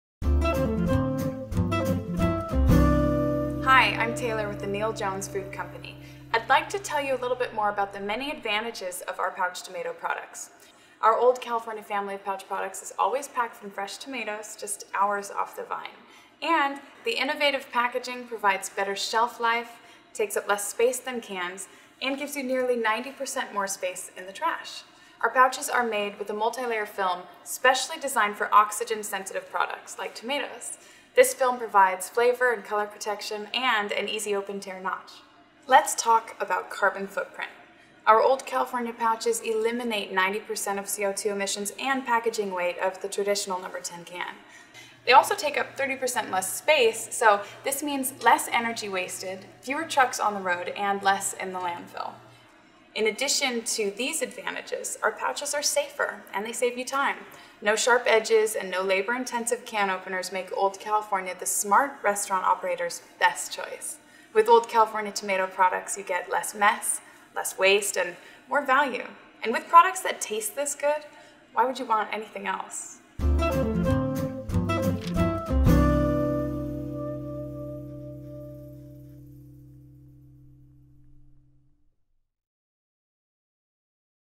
Old California Pouch Benefits
Watch and learn about the advantages of the Pouch Tomato products from Neil Jones Food Company.  From better shelf life to reducing your carbon footprint.  Watch to learn more.